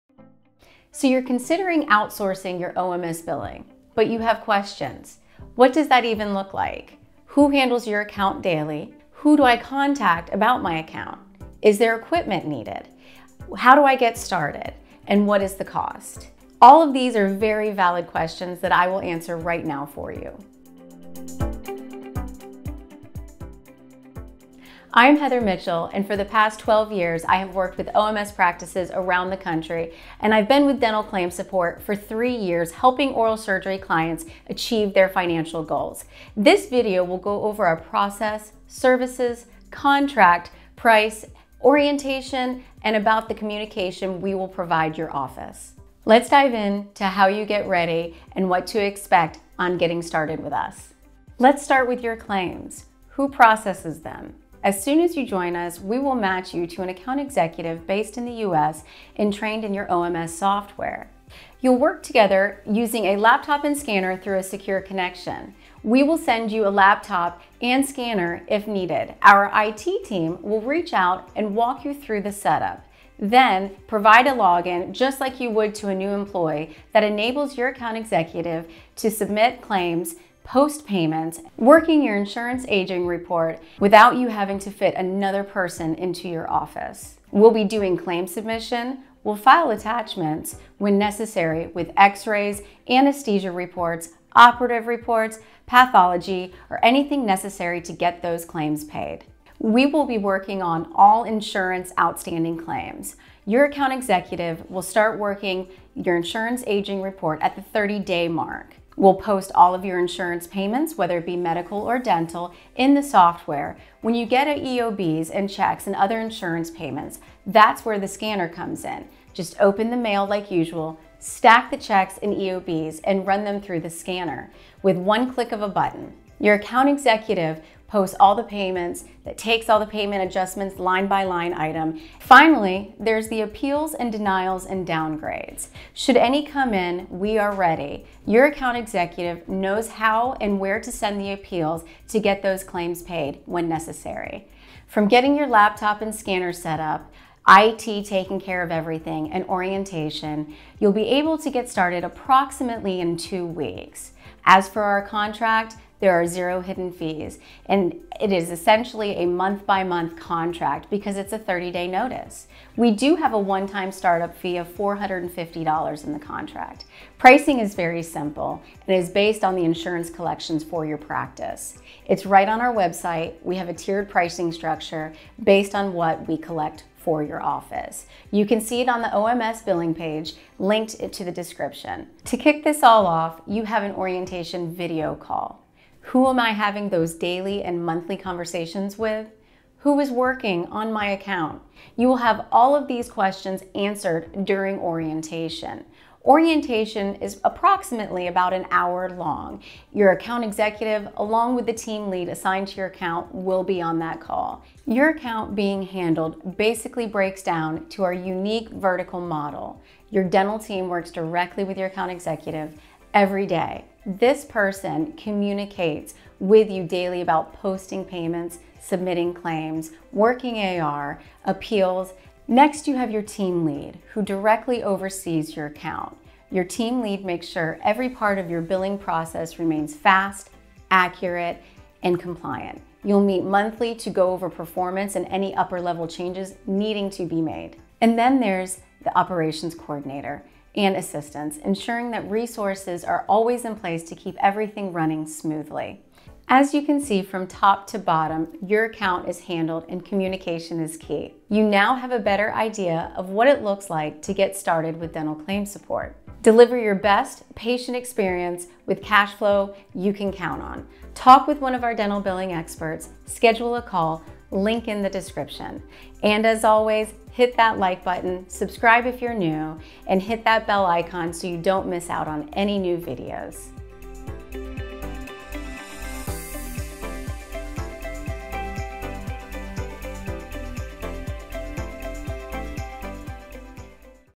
Understand the Outsourced OMS Billing Process - Easy Oral and Maxillofacial Surgery Billing!
In this video:
0:00 Intro
0:24 OMS Billing - Dental ClaimSupport
0:51 Process & Services
2:24 How Fast Can We Start?
2:34 Contract
2:47 Pricing
3:02 Orientation & Communication
3:25 Who Works On Your Account
4:16 Schedule a call!

Understanding oral and maxillofacial surgery billing is important to collecting payment quickly and properly. We will walk you through a clear picture of how OMS billing works at DCS and our process. And how to bring in more revenue from both patients and medical insurance claims.

OMS billing is more than a system to collect money.
It's the health of your billing system that will make or break your oral and maxillofacial surgery practice.
And understanding the OMS billing process is how you improve and maintain that health. This process is a must know for oral and maxillofacial surgery offices!

These are commonly asked questions that we get all the time about working with an outsourced dental billing company!